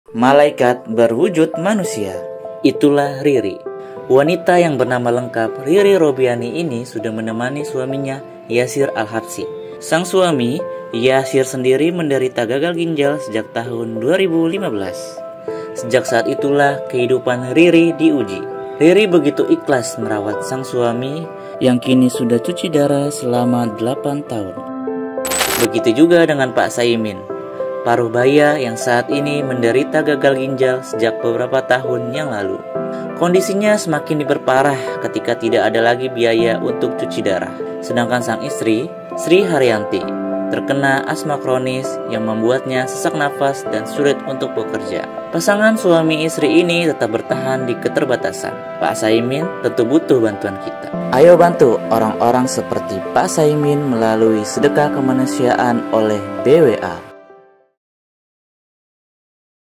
Bantu Perjuangan Bapak Saimin dan Istrinya Berobat
Orang bilang, ketika sudah tua, berbagai penyakit akan mulai berdatangan. Barangkali begitulah yang terjadi pada Pak Saimin dan istrinya. Kini mereka sudah tidak muda lagi dan keduanya pun memiliki penyakit yang harus rutin diperiksakan.

Tidak hanya Pak Saimin, masih banyak masyarakat lain yang juga menderita sakit parah dan membutuhkan biaya yang tidak sedikit. Entah itu orang tua atau anak-anak. Sayangnya, mereka tidak memiliki cukup dana hingga proses pengobatan terkendala.

Oleh sebab itu, demi membantu masyarakat yang sakit dan membutuhkan biaya, BWA mengajak semua orang untuk meringankan beban mereka. Bantuan yang diberikan akan sangat berarti demi kesembuhan saudara kita.

Bantuan Anda bahkan dapat memberi harapan kesehatan pada mereka yang sakit. Mari bantu bersama!